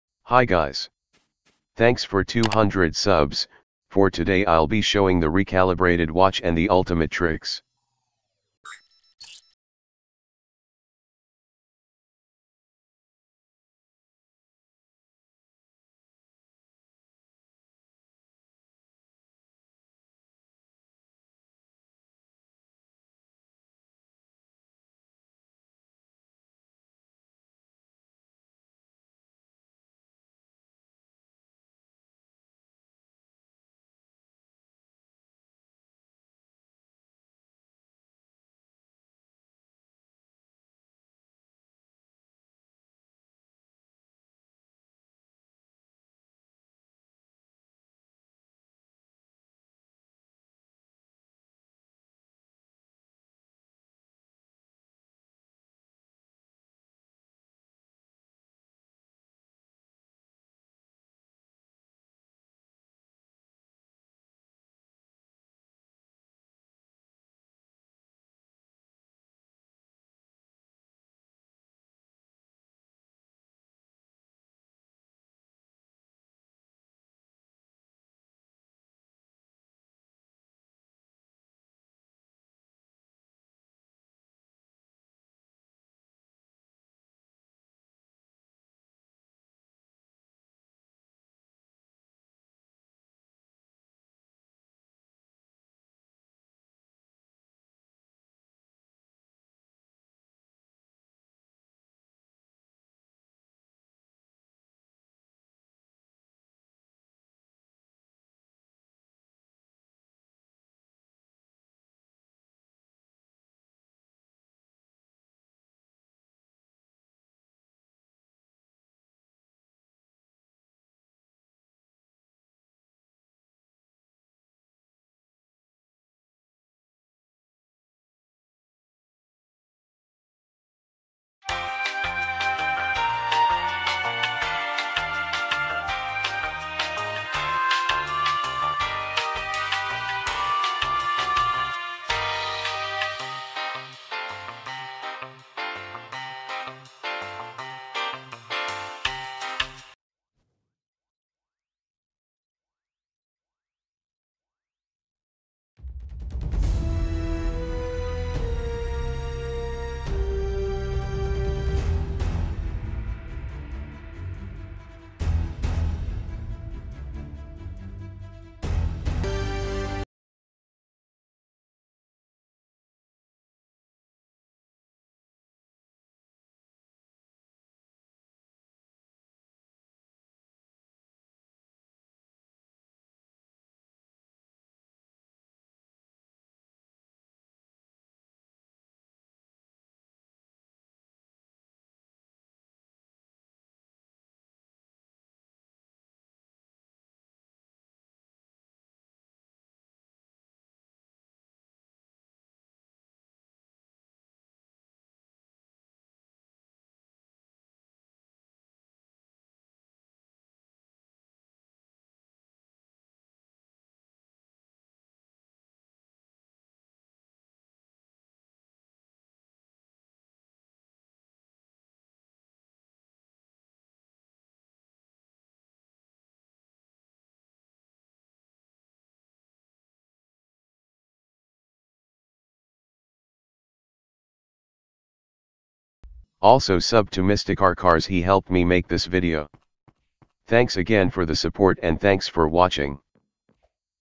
AF watch & Ultimatrix! | PrisonerChives
I showed how to get the recalibrated and ultimatrix watch in the video and also showed their features!

Timestamps:
0:00 - Intro
0:08 - Best way to farm points
1:19 - Upgrading Watches
2:56 - Features
3:55 - Outro